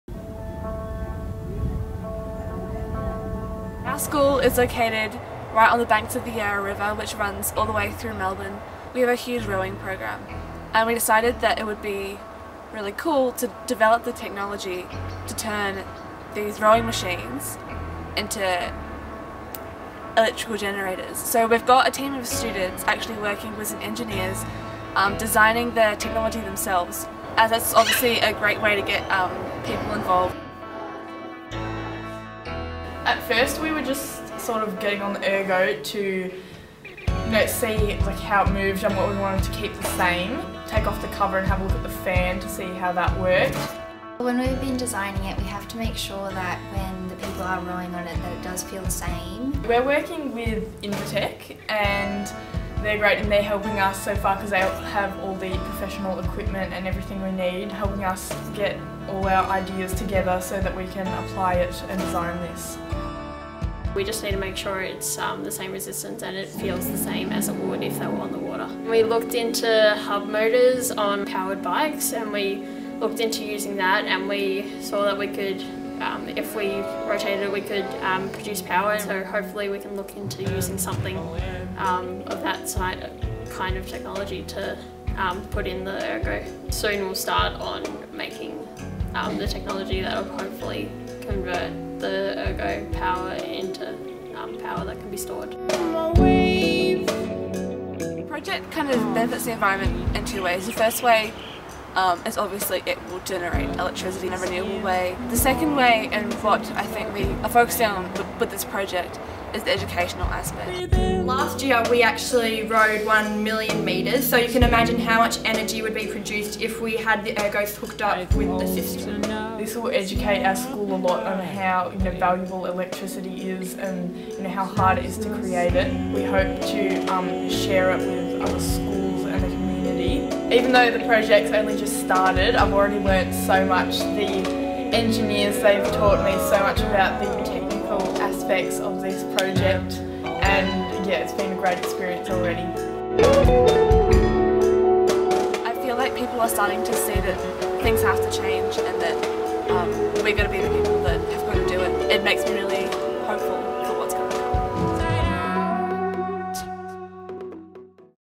Ergo Update 1
Progress report on MGC's ZFEP-funded rowing machine project!